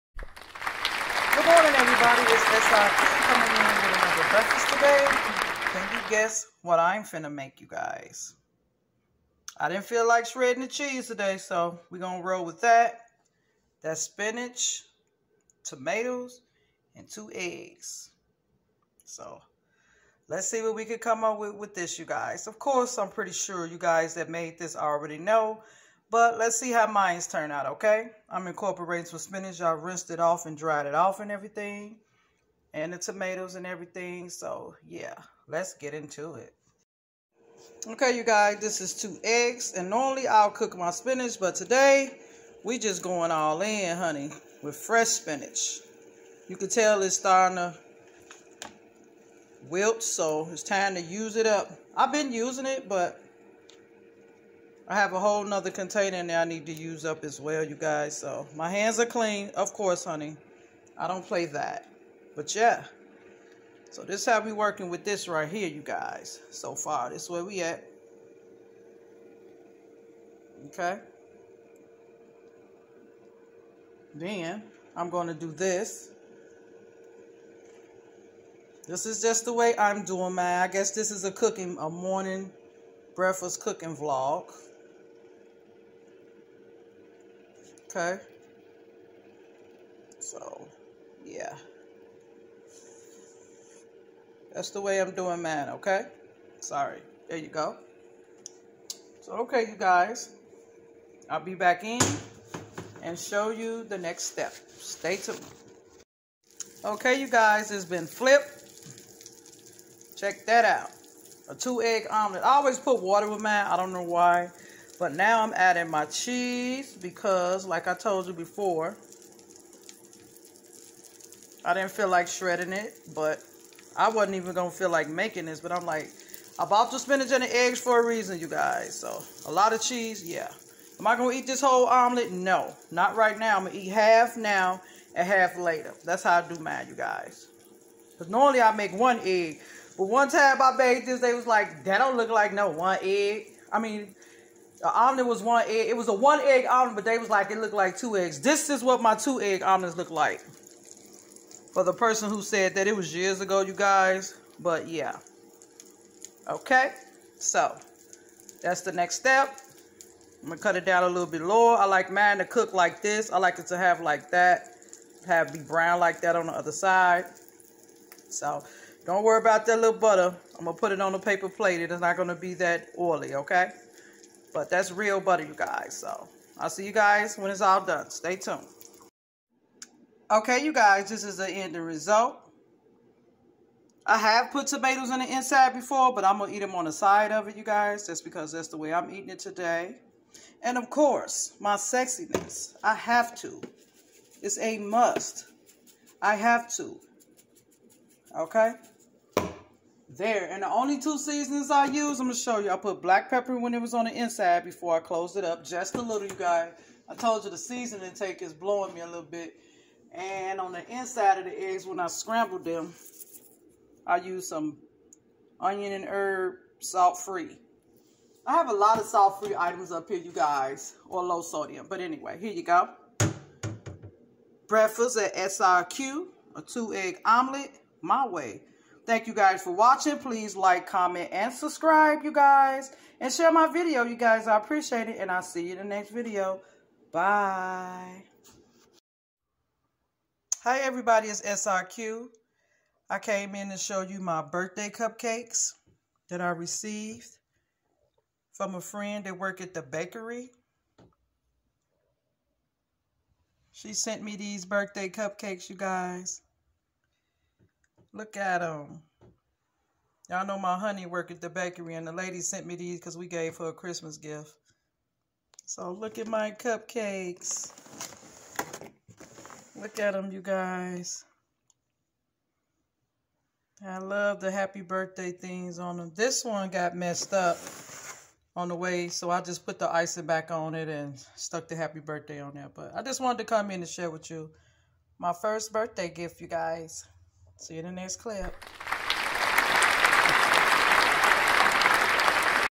Kickstart Breakfast With SRQ
Sharing My way of eating an healthy Breakfast..Spinach..Also sharing my cupcakes that was giving to me for an early Birthday gift 🎁..TFW